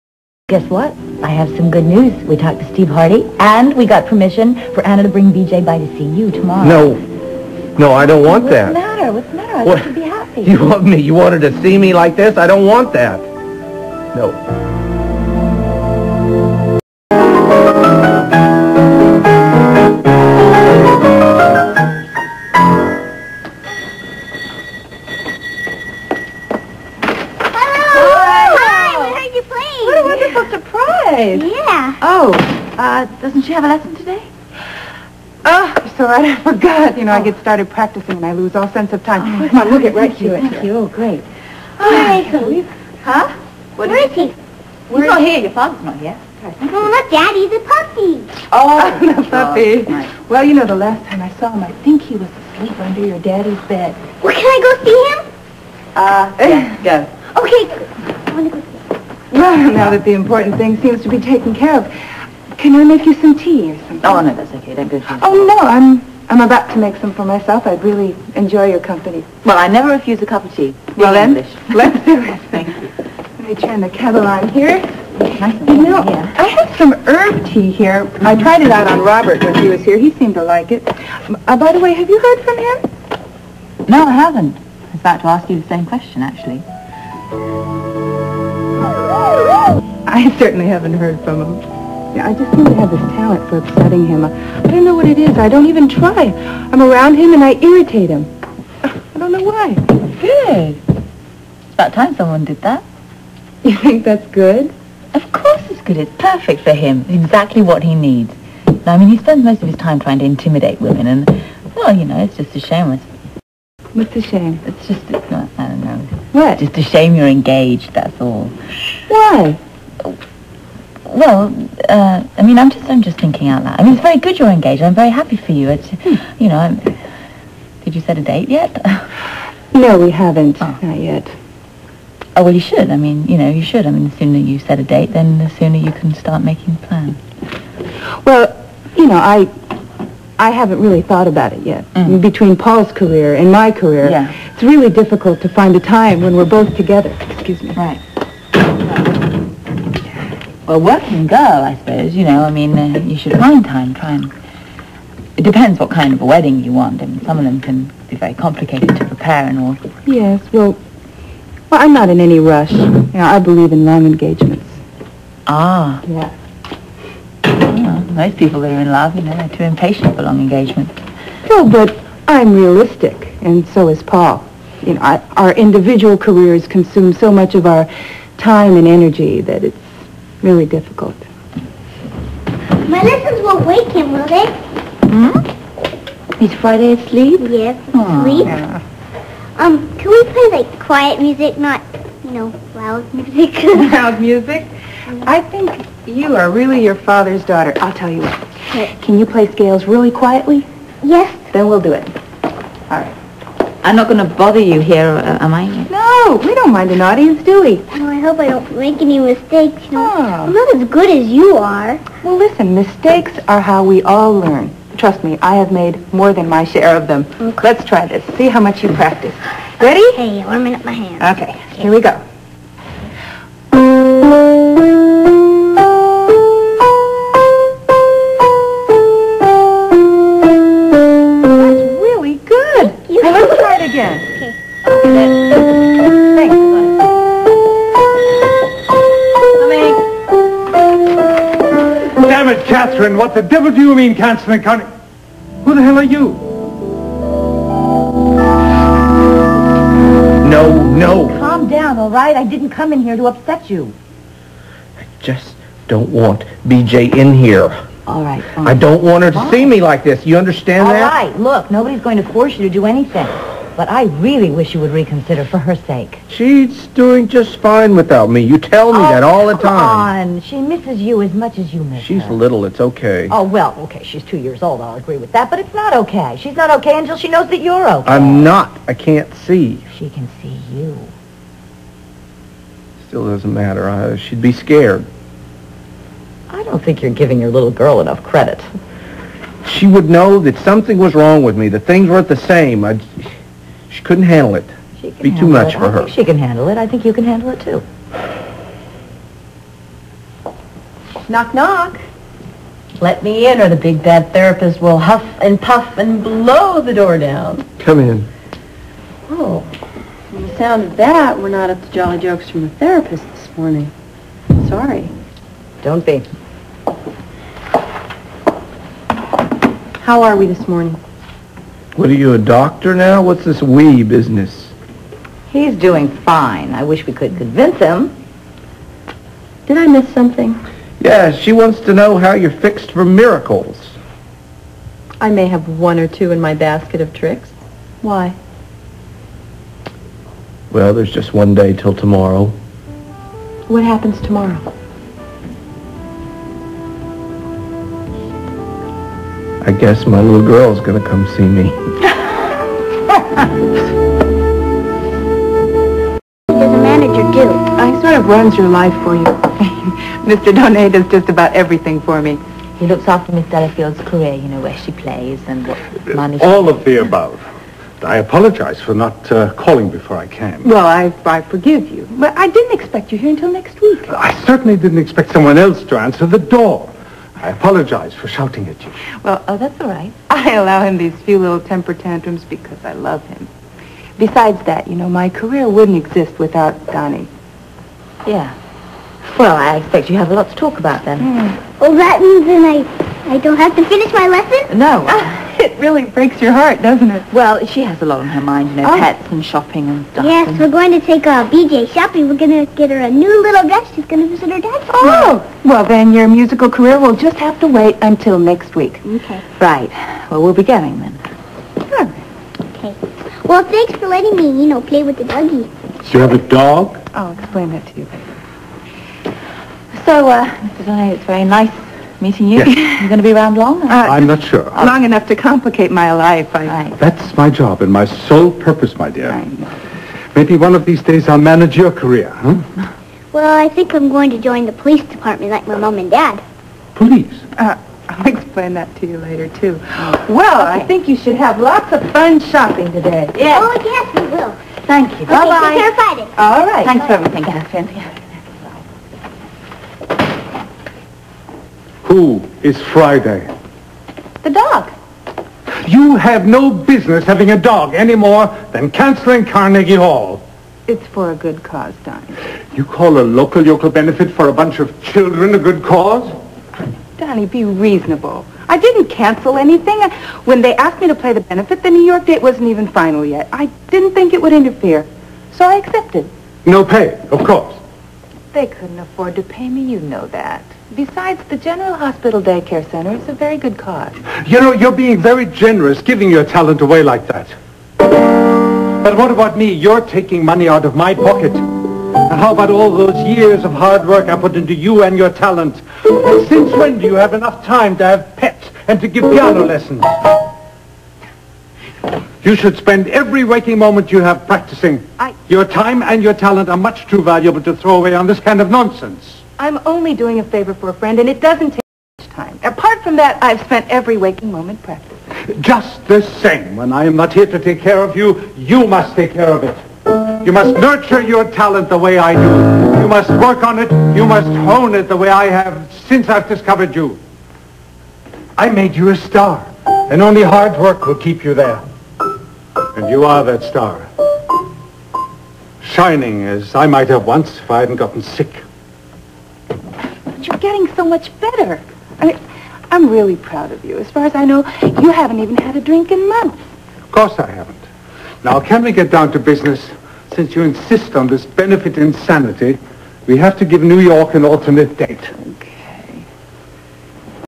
BJ visits her Daddy 1
Guess what? I have some good news
We talked to Steve Hardy and we got permission for Anna to bring BJ by to see you tomorrow.
No No I don't want that.
What's the matter? What's the matter? I thought you would be happy.
You want me...you want her to see me like this? I don't want that. No
Follows "Robert's surprise party"